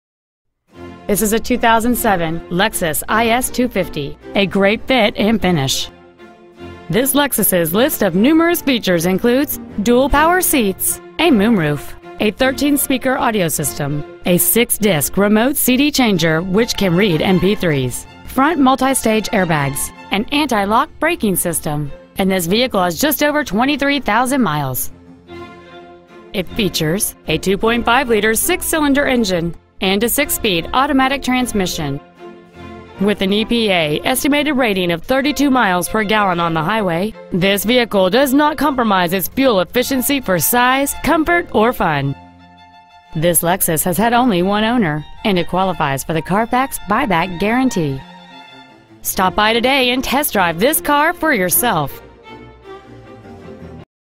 Preowned 2007 Lexus IS 250 Concord CA
Year: 2007
 Make: Lexus
 Model: IS 250
 Engine: 2.5L V-6cyl
 Trans.: 6-Spd Auto Overdrive
 Exterior: Silver
 Miles: 23,069
 Interior: Gray